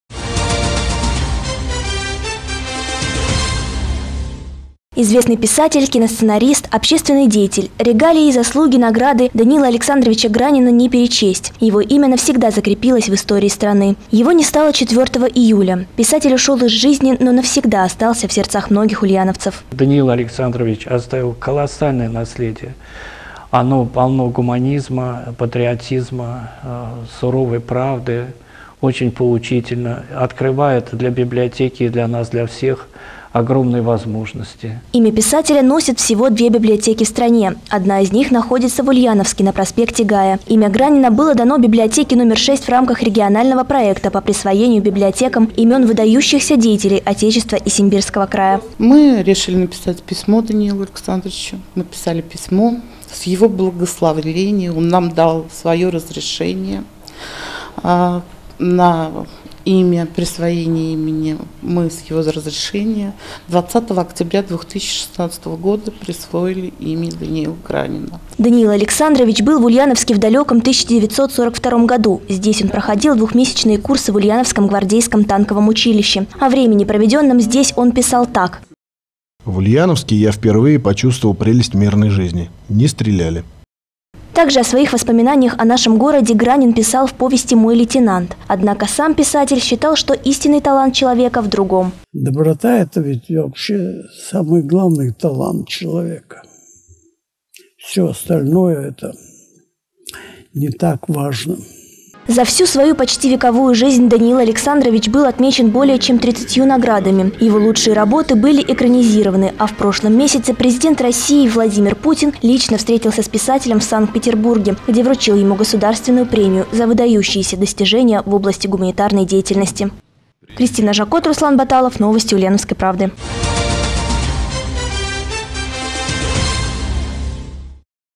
Даниил Гранин: «В Ульяновске я впервые почувствовал прелесть мирной жизни. Не стреляли»
Известный писатель, киносценарист, общественный деятель: регалии, заслуги и награды Даниила Александровича Гранина не перечесть. Его имя навсегда закрепилось в истории страны. Его не стало 4-го июля, писатель ушёл из жизни на 99-ом году жизни, но навсегда остался в сердцах многих россиян, в том числе и ульяновцев.